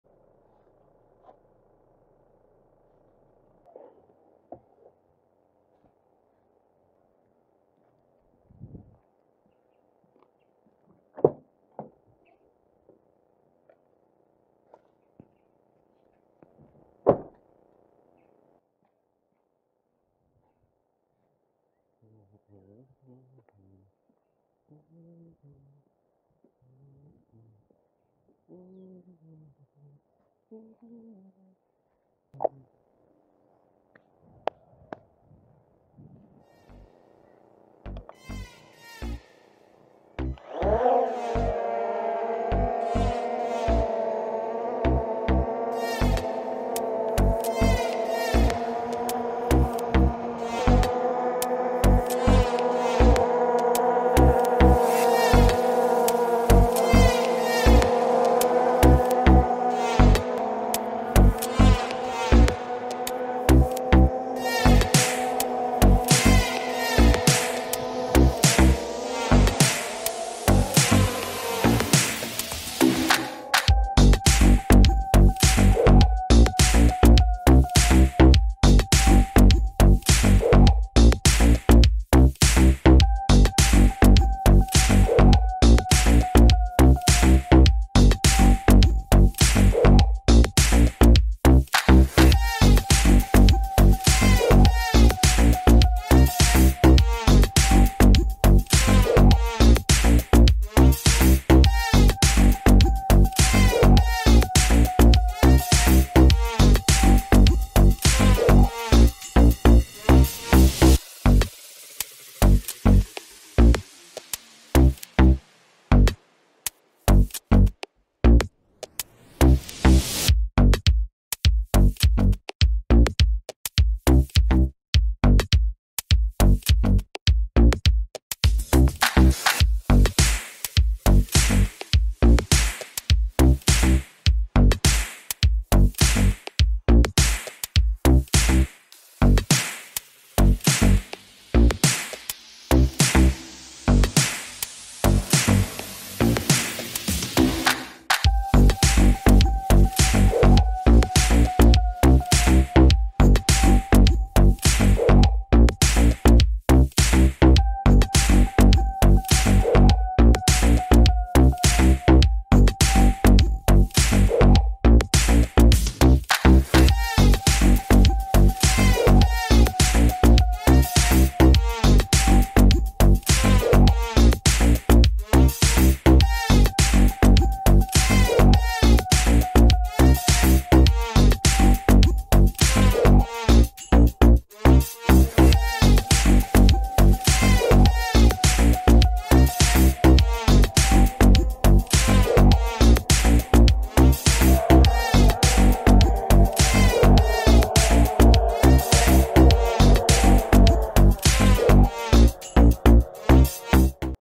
MJXRC BUGS20 EIS VIDEO FOOTAGE (B20 EIS)
Hello Evrybuddy. Flying B20's and recorded quality videos with 4k's resolution.